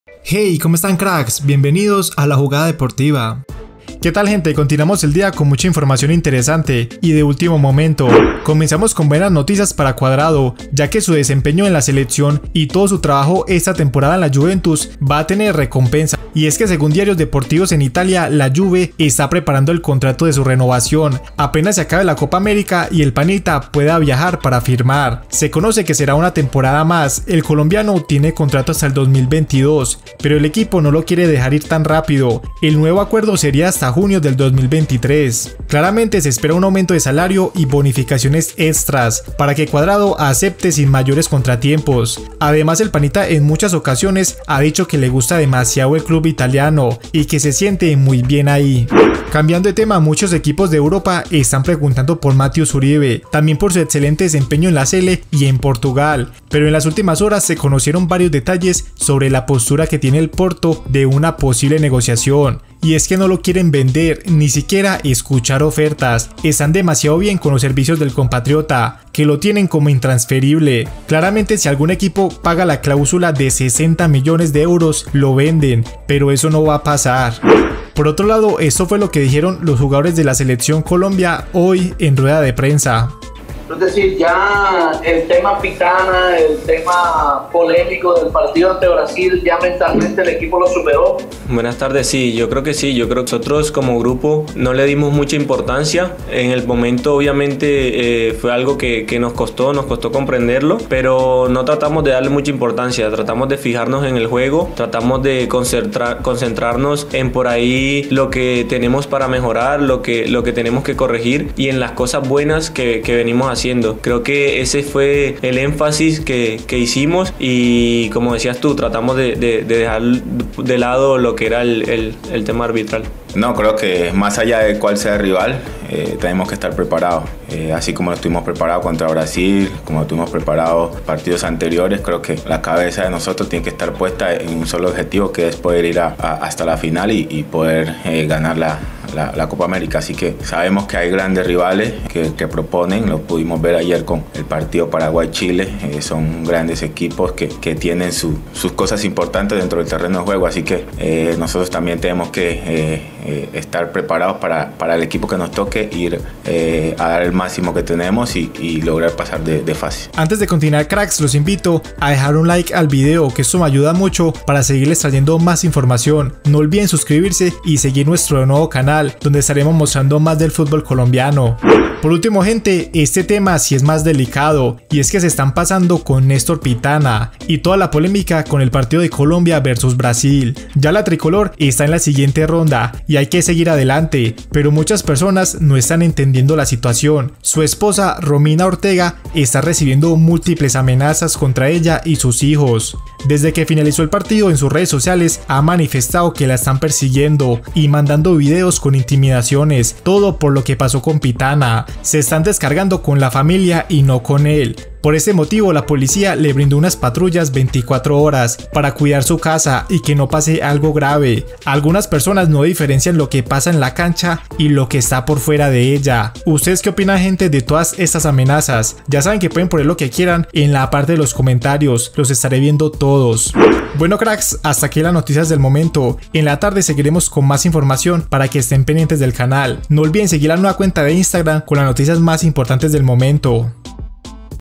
POR ESTO POLICIA ESTA EN LA CASA DE PITANA | BORRE SOBRE EL ROBO | CUADRADO CONTRATO
RESUMEN

1. Comenzamos con buenas noticias para Cuadrado ya que su desempeño en la selección y todo su trabajo esta temporada en la Juventus va a tener su recompensa, preparan contrato de renovación hasta el 2023.

2. Muchos equipos de Europa están preguntando por Mateus Uribe, también por su excelente desempeño en la Selección y en Portugal.

3. Rueda de prensa Santos Borre y Fabra.

4. Grave situación con Néstor Pitana y su familia por la polémica del partido Colombia vs Brasil.